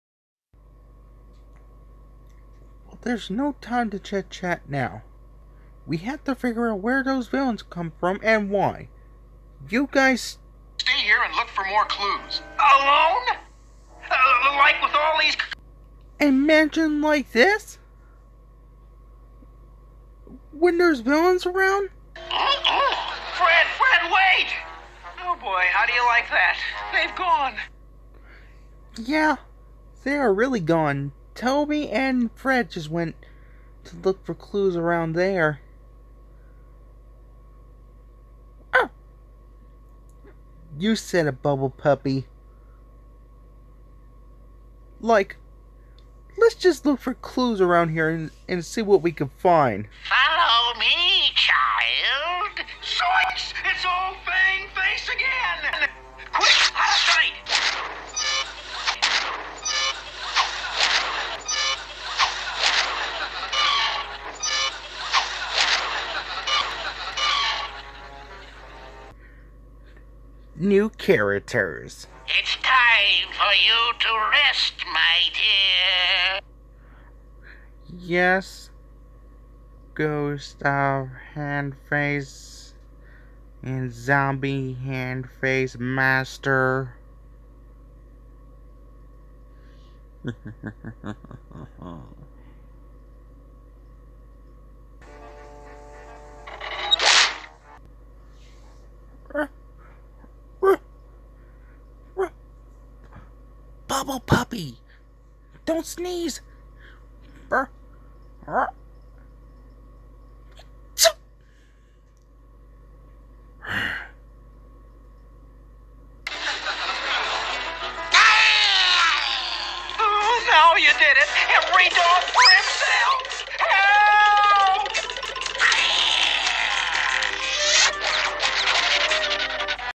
More And More Villains Appear Everywhere Part 5a Out Of 5
New Characters They Are Captured By The Ghost Of Hand Face And The Zombie Hand Face: TheGlorpCorp TheMunchos TheFrosticons MrGrumpfish And Sticks Themselves
For Jorge Cruz And Callie Squid Sister